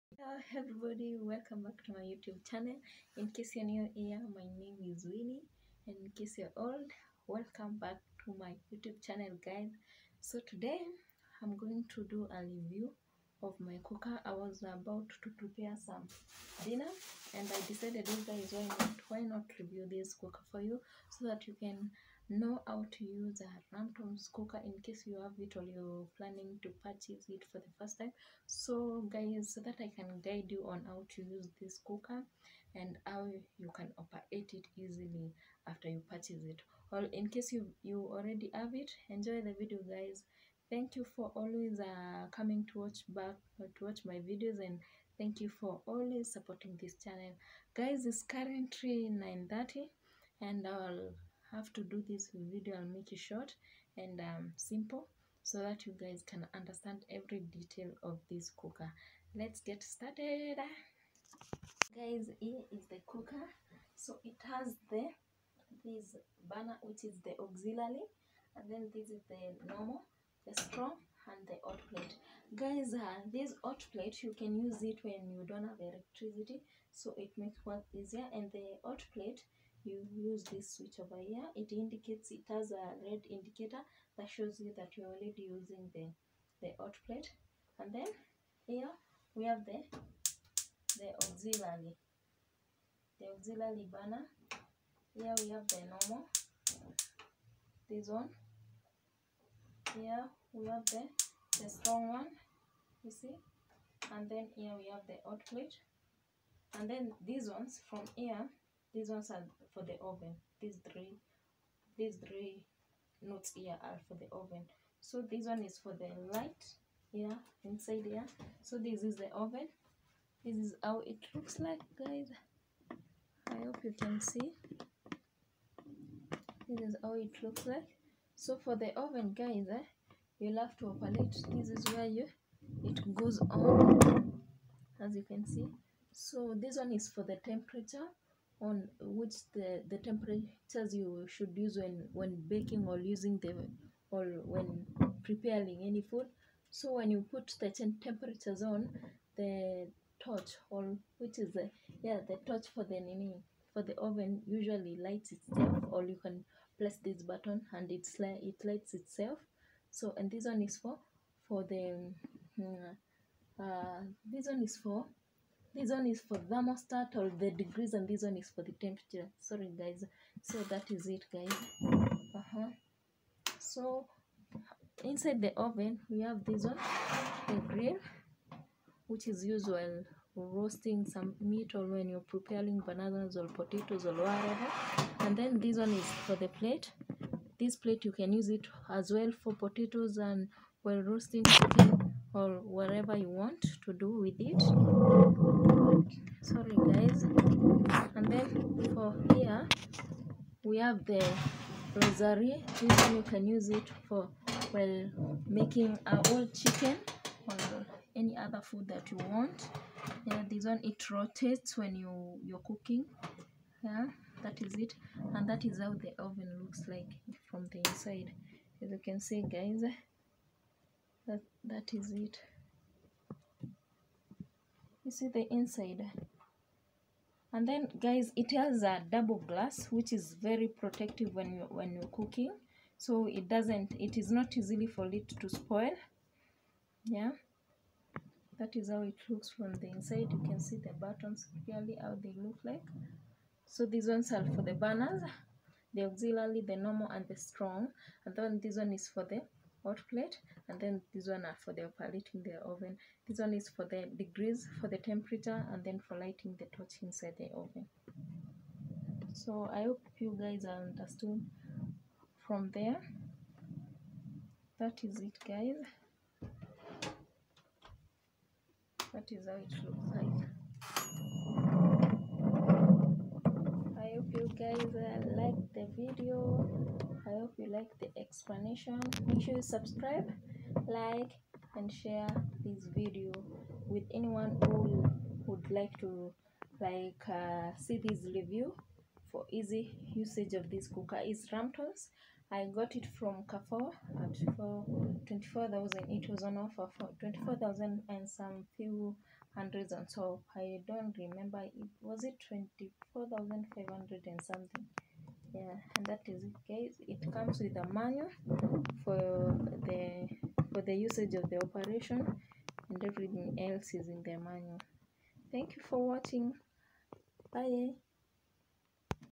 Ramtons Cooker Review|How to use the Cooker+Oven|VerySimple & Detailed 3 Burners +1Hotplate#episode2
Hi everybody here is a detailed review of my Cooker incase you want to know more about this watch the whole video.